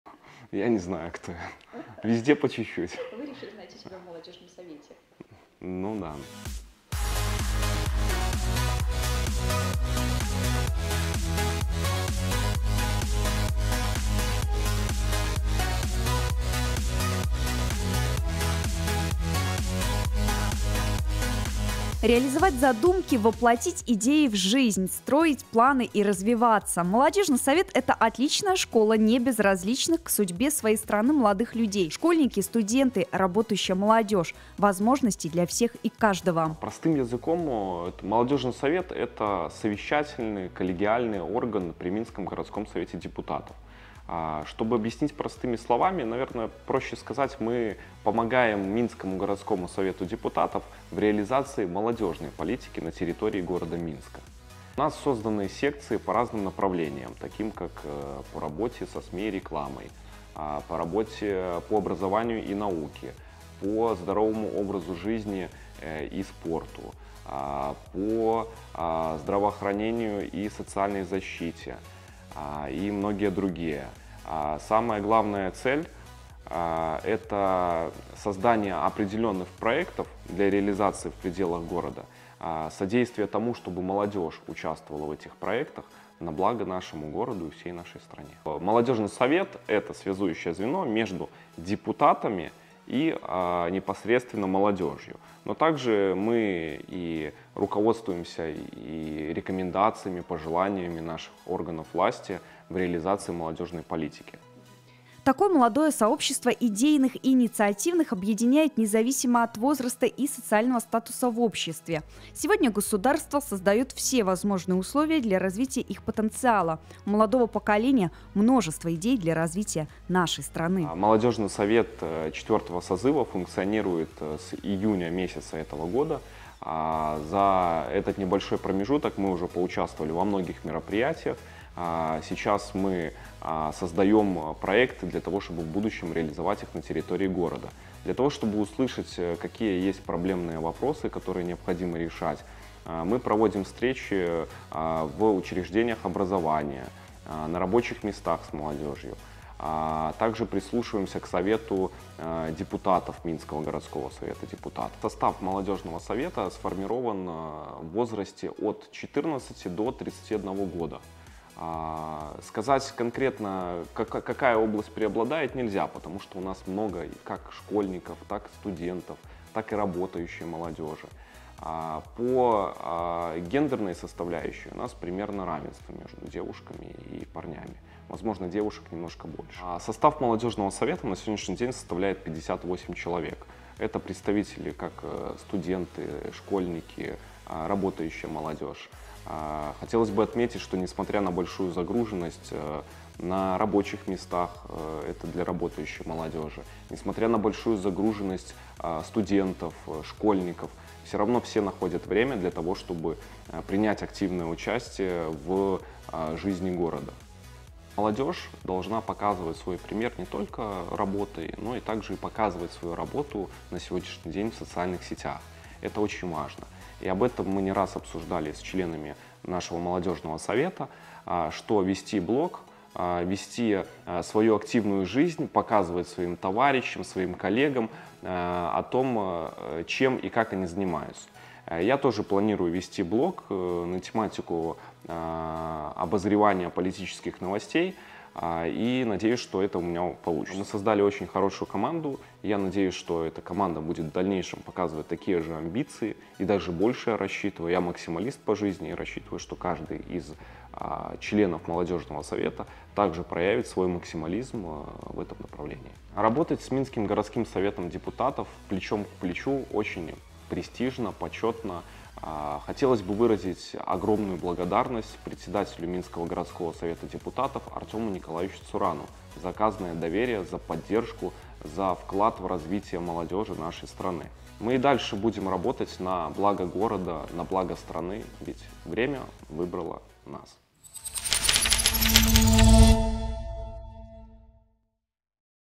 Совет молодежи - вступай в Молодежный совет || Залог успеха - сплоченность команды!
Четвертый созыв Молодежного совета при Минском городском Совете депутатов начал функционировать в июне этого года и уже успел зарекомендовать себя среди молодежи.  Почти 60 молодых людей из разных сфер объединились, чтобы сделать нашу страну еще лучше. О целях, проектах и жизни совета рассказал его председатель Владислав Демиденко.

Над сюжетом работали Ксения Грузинова, Юрий Ткаченко.